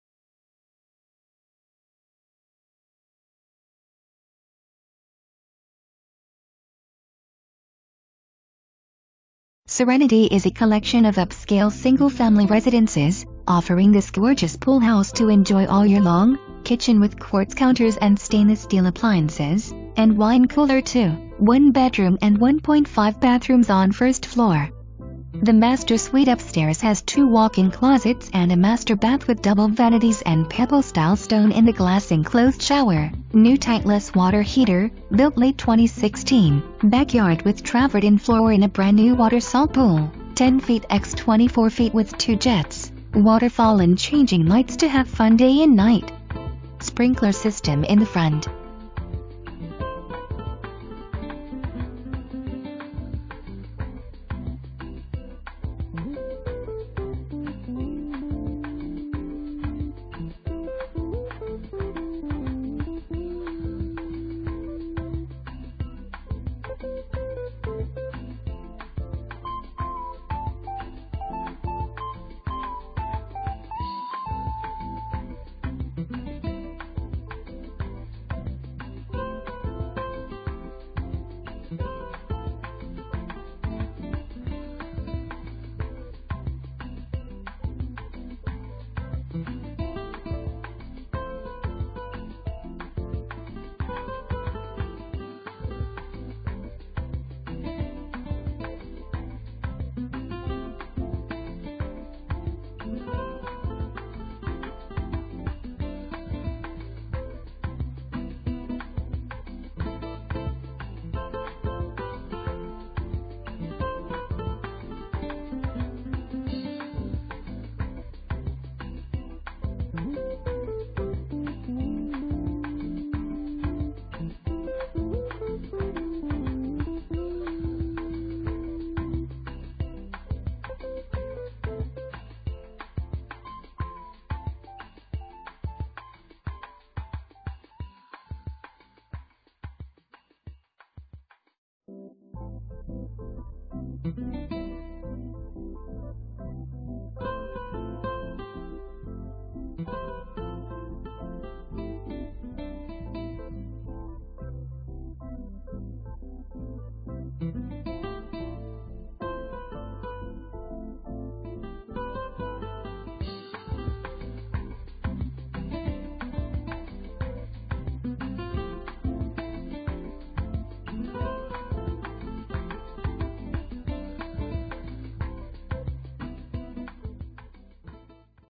Homes for Sale - 15973 SW 136th Way, Miami, FL
Serenity is a collection of upscale single-family residences, Offering this Gorgeous Pool House to enjoy all Year long, kitchen with quartz counters and Stainless Steel appliances, and wine cooler too, 1 bedroom and 1.5 bathrooms on first floor. The master suite upstairs has 2 walk-in closets and a master bath with double vanities and pebble style stone in the glass-enclosed shower, New Tank less Water Heater, Built late 2016, Backyard with travertine floor and a Brand New Water Salt Pool, 10' X 24' with 2 jets, Water Fall and changing lights to have fun day and night. Sprinkler system in the front.

Presented By:
Victoria Pando, Berkshire Hathaway HomeServices Florida Realty